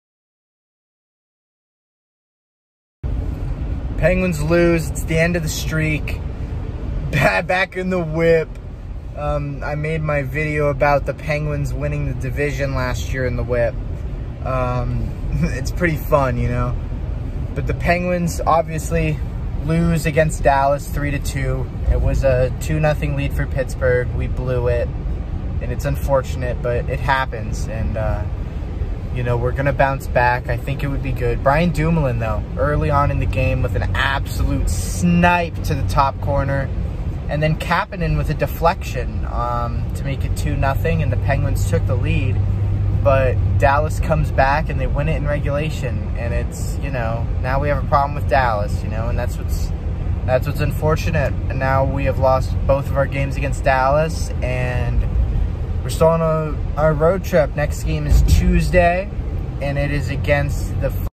Game 34: Penguins lose 3-2
Scores
1st period
PIT - B. Dumoulin (K. Letang, J. Guentzel)
PIT - K. Kapanen (M. Pettersson, J. Carter)
2nd period
DAL - D. Gurianov (J. Peterson, J. Robertson)
3rd period
DAL - J. Pavelski (J. Robertson, J. Klingberg)
DAL - R. Hintz (M. Heiskanen, J. Robertson)